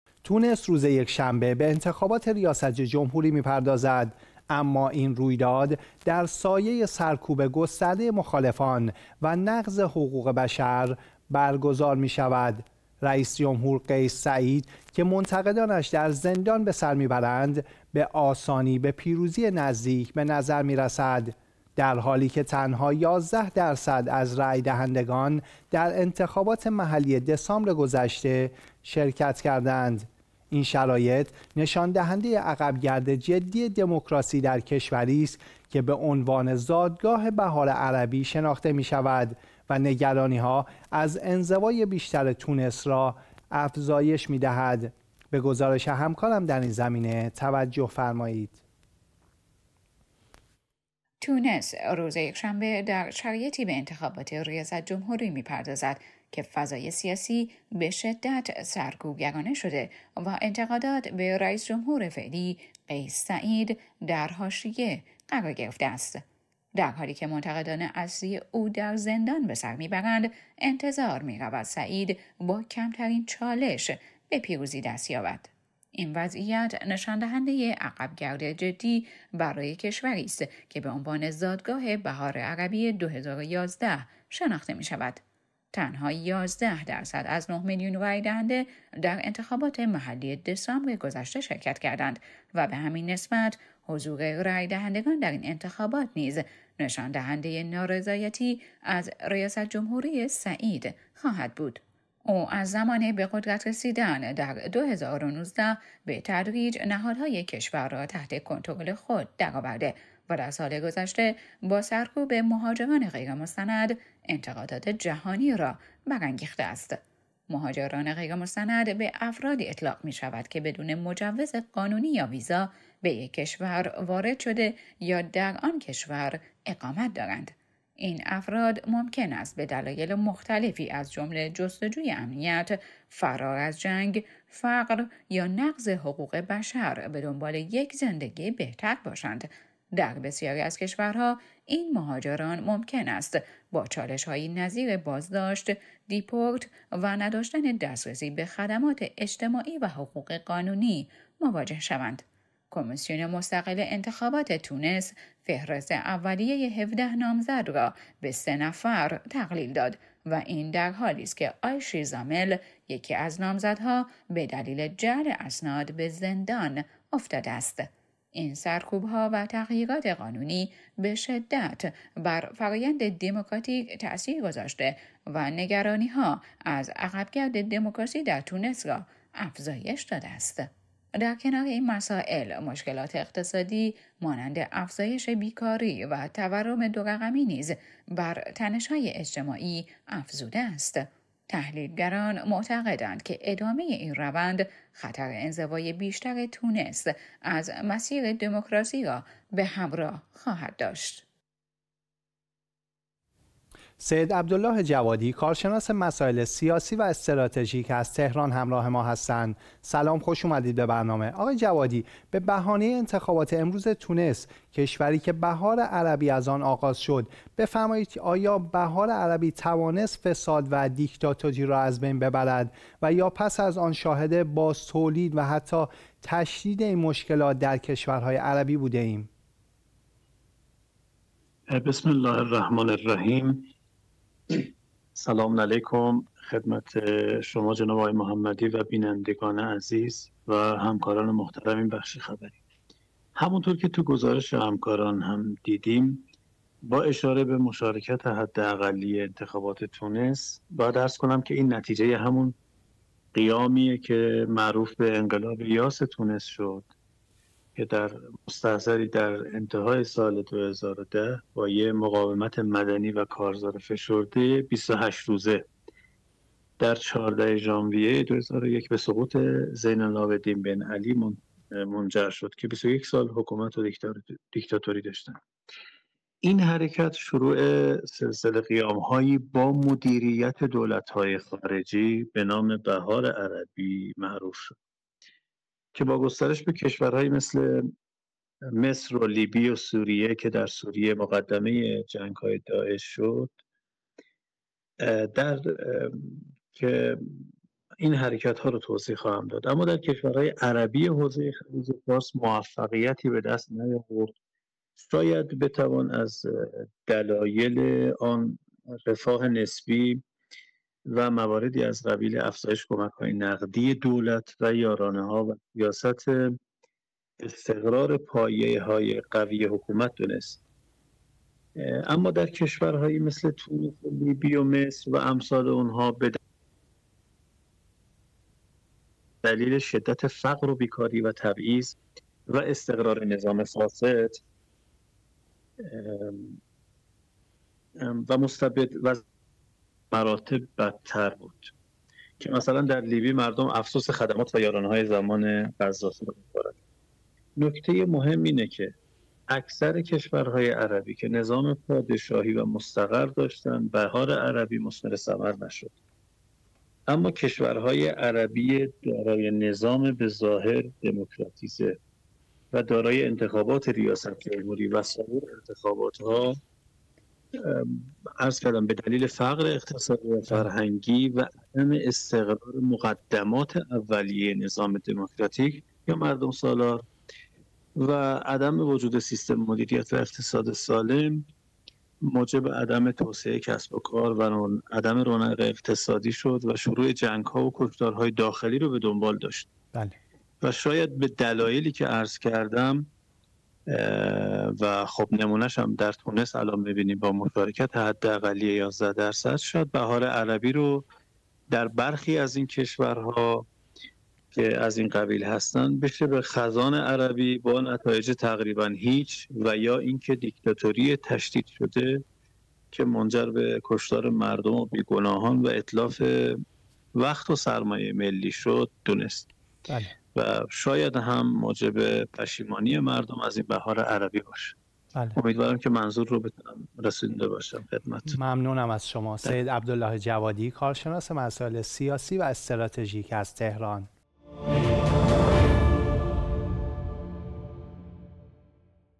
انتخابات تونس در سال 2024، در سایه سرکوب مخالفان و نقض حقوق بشر
انتخابات تونس در سال ۲۰۲۴، در سایه سرکوب مخالفان و نقض حقوق بشر برگزار می‌شود. رئیس‌جمهور قیس سعید با سرکوب منتقدان و مخالفان خود، فضایی به شدت محدود برای رقابت‌های انتخاباتی ایجاد کرده است.
حضور کم‌رنگ رأی‌دهندگان در انتخابات‌های گذشته، به‌ویژه شرکت تنها ۱۱ درصد از رأی‌دهندگان در انتخابات محلی دسامبر گذشته، نشان از نارضایتی عمومی دارد.
 این انتخابات نشان‌دهنده عقب‌گرد دموکراسی در تونس است، کشوری که به‌عنوان مهد بهار عربی شناخته می‌شود و اکنون با بحران سیاسی و اجتماعی مواجه است.

انتخابات تونس به عنوان رویدادی کلیدی در مسیر آینده این کشور شناخته می‌شود. قیس سعید، که از سال ۲۰۱۹ به قدرت رسیده است، توانسته با اعمال فشار بر نهادهای سیاسی و سرکوب منتقدان، قدرت خود را تقویت کند.
 این وضعیت نگرانی‌های گسترده‌ای را درباره انزوای بیشتر تونس و کاهش دستاوردهای دموکراتیک بهار عربی ایجاد کرده است. از سوی دیگر، مشکلات اقتصادی نظیر بیکاری و تورم دو رقمی نیز تنش‌های اجتماعی را در این کشور تشدید کرده‌اند.

ویدیوهای مرتبط